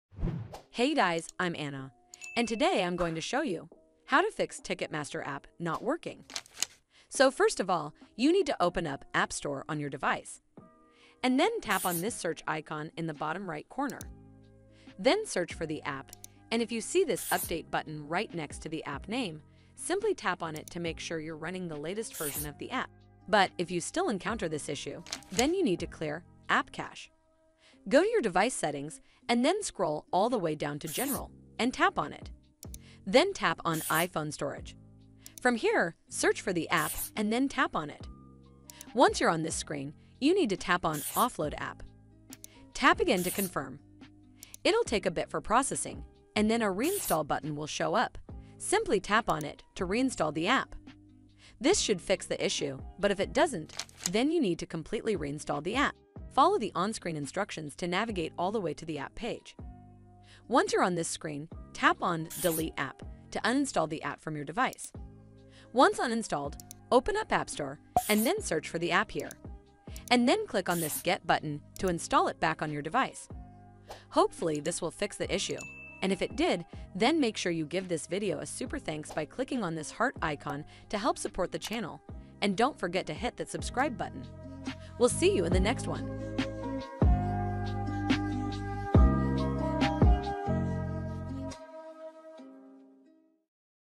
Ticketmaster App Not Working: How to Fix Ticketmaster App Not Working
In this video, I'll show you How to Fix Ticketmaster App Not Working. This is the easiest and fastest way to Fix Ticketmaster App Not Working. Make sure you watch until the end of this video to find out How to Fix Ticketmaster App Not Working on Android and iPhone. These methods work on Android as well as iOS 11, iOS 12, iOS 13 and iOS 15. Hope you enjoy!

Video Parts:
00:00 Intro: How to Fix Ticketmaster App Not Working
00:07 Method 1: Updating Ticketmaster App
00:25 Method 2: Clearing Ticketmaster App Cache
01:13 Outro: Conclusion

why are my tickets not showing on ticketmaster app